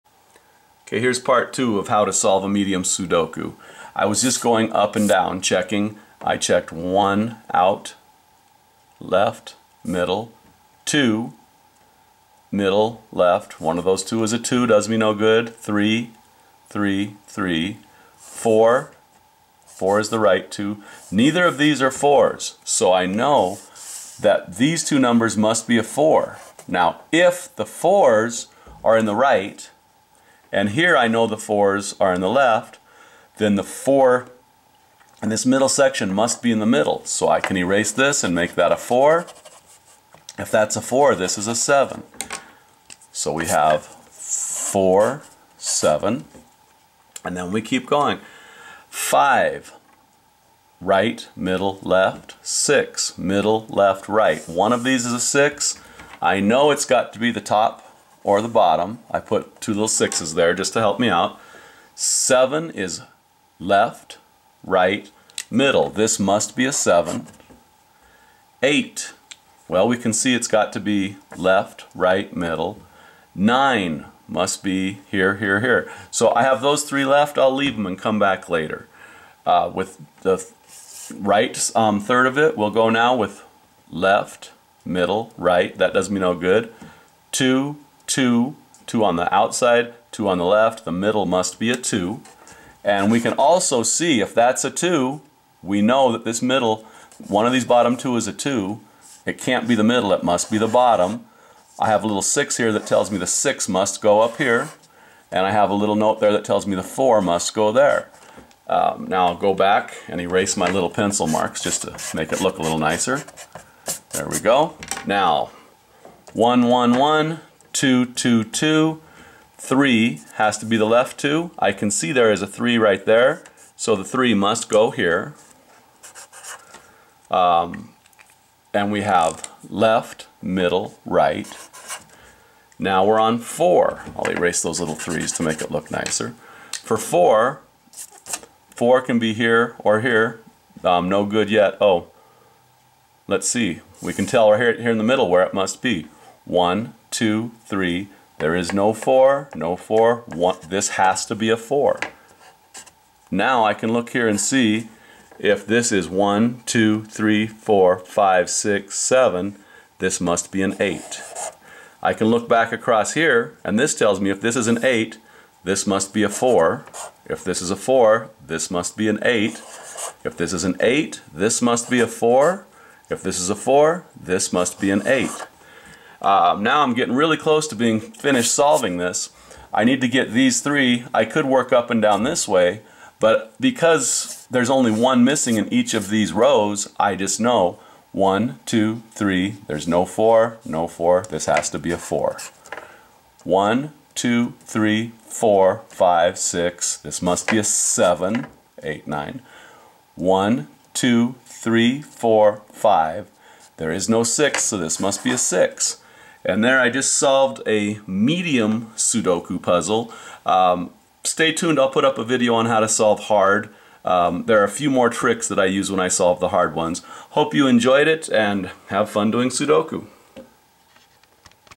How to Solve Medium Sudoku Puzzles (Pt. 2)
Here's the second part of my How to Solve Medium Sudoku Puzzles video.  I hope you find it helpful.